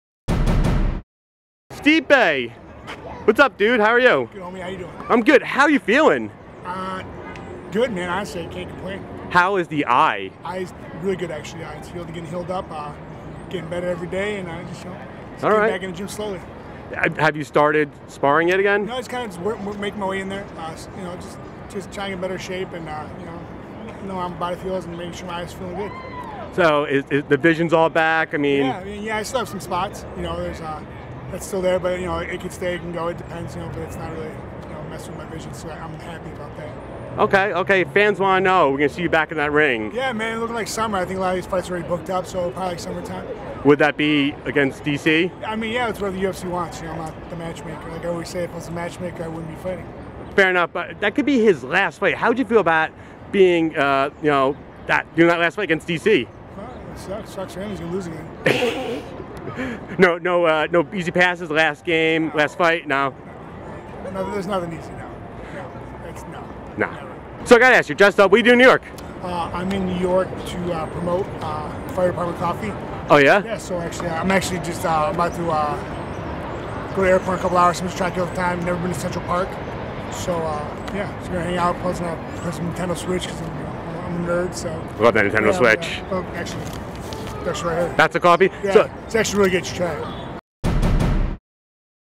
Stipe Miocic's Eye Is 'Really Good,' Targeting Summer Fight with Daniel Cormier | TMZ Sports
Great news for Stipe Miocic ... the UFC heavyweight champ says his injured eye is healing up really well ... and he expects to fight Daniel Cormier in just a few months!